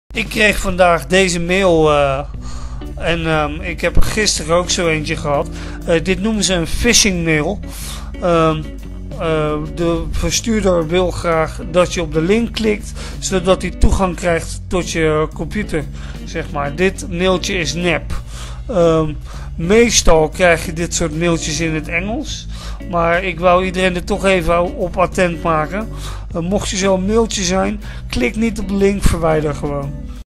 LET OP! PHISHING MAIL - Mediamarkt
Ik ben toch niet gek! :D

Phishing (naar analogie van phreaking, is het afgeleid van fishing: "vissen", "hengelen") is een vorm van internetfraude.

Hey leuk dat je kijkt naar mijn video! Vergeet niet om deze video te LIKEN en kijk ook eens op mijn